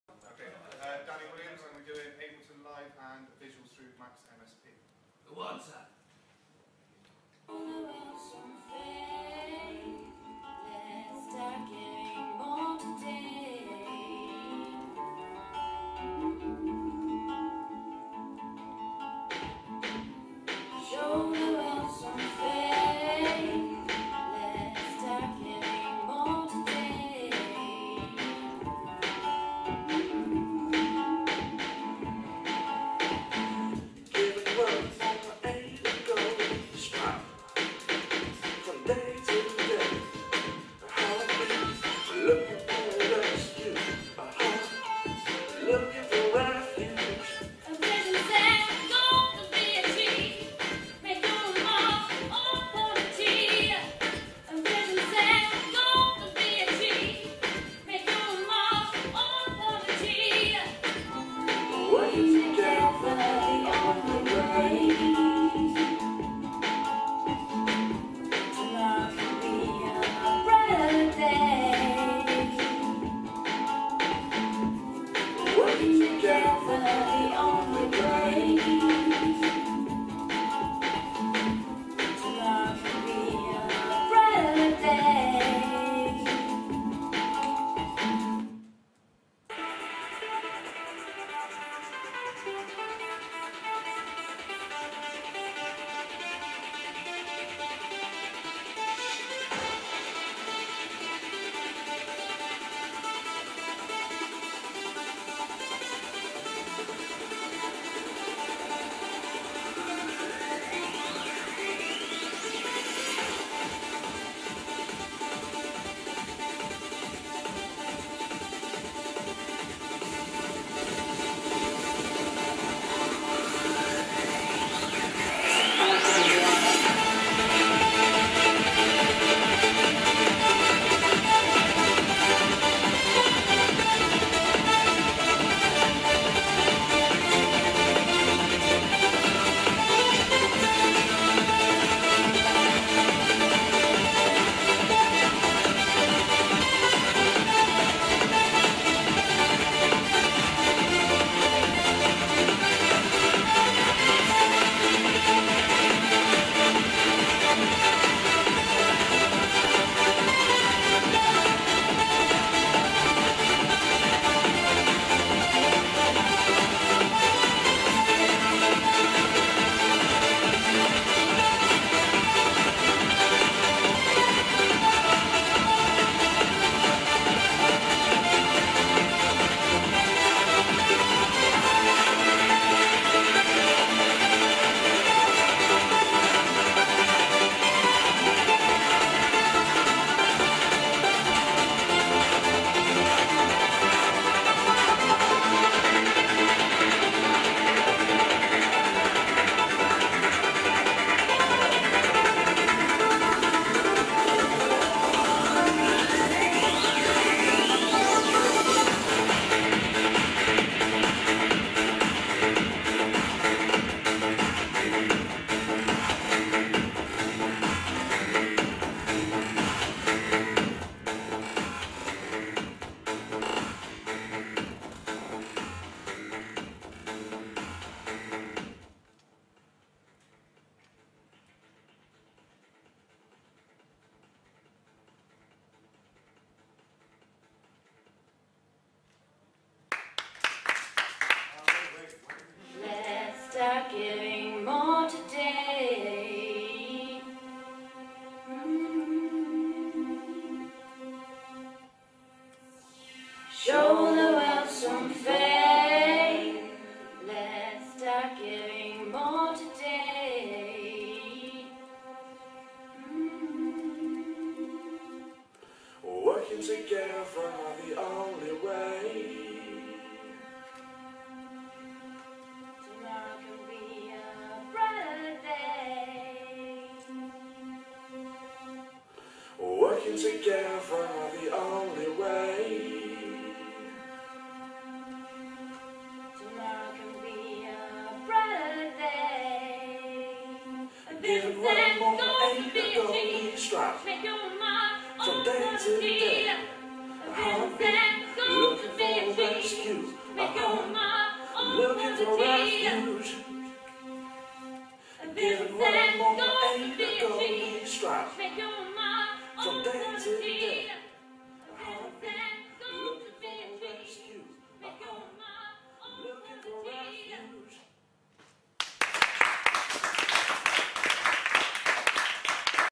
FdA Music Production @ City of Bath College (2)
Students showcase Technology in Performance work